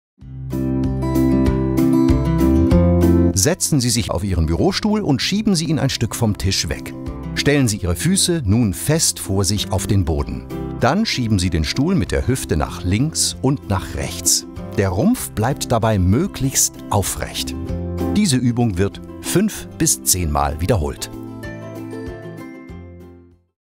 Officing Übung 3 – Orthomol arthroplus
Die meisten von uns verbringen im Schnitt bis zu 14 Stunden am Tag im Sitzen – und davon die meiste Zeit am Schreibtisch. Auf Dauer kann das für unsere Gelenke eine ganz schöne Belastung werden. Um auch im Büro Bewegung in den Alltag zu bringen,
gibt es eine Reihe von hilfreichen Büro-Übungen, die sich leicht in den Tagesablauf integrieren lassen.

So geht Übung 3: Setzen Sie sich wieder auf Ihren Bürostuhl und schieben Sie ihn ein Stück vom Tisch weg. Stellen Sie Ihre Füße nun fest vor sich auf den Boden. Dann schieben Sie den Stuhl mit der Hüfte nach links und nach rechts. Der Rumpf bleibt dabei möglichst aufrecht. Auch diese Übung wird 5-10 mal wiederholt.